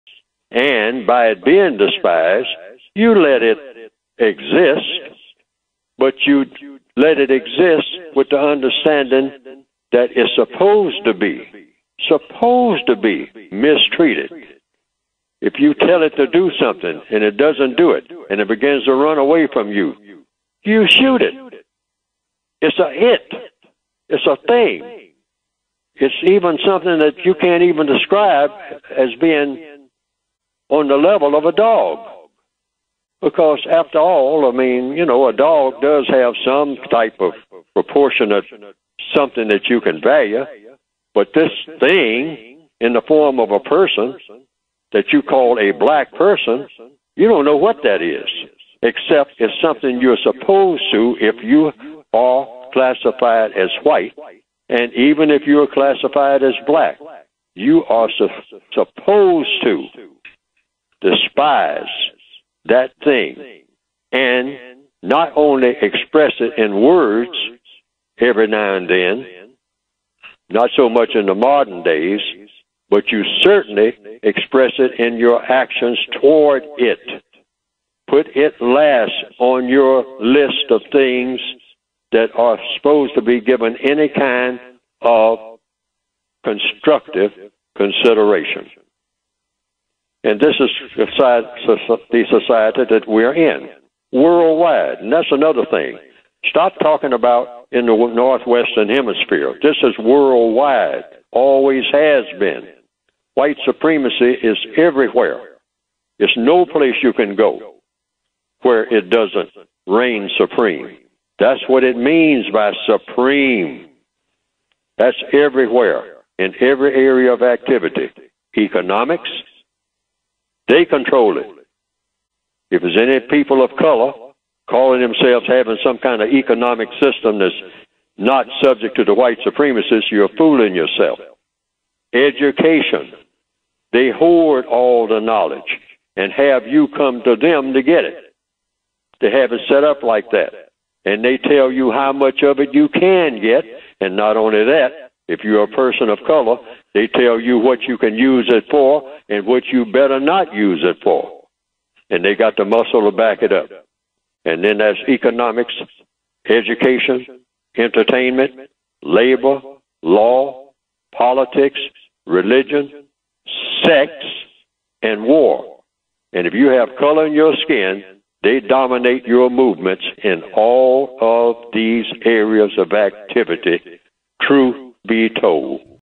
Neely Fuller- They Control Everything Everywhere
Neely Fuller jr explains that anybody who thinks white supremacy does not reign supreme everywhere on the planet they are fooling themselves.